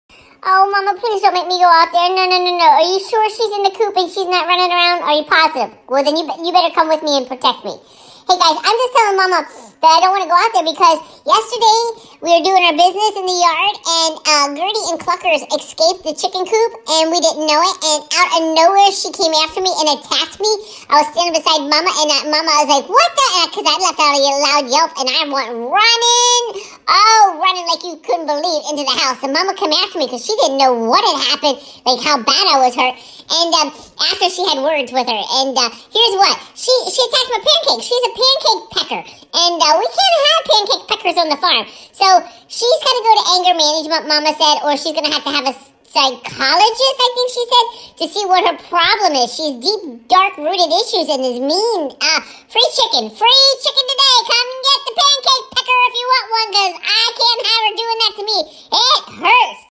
Pancake Pecker
Gabby Girl got attacked by a pancake pecker.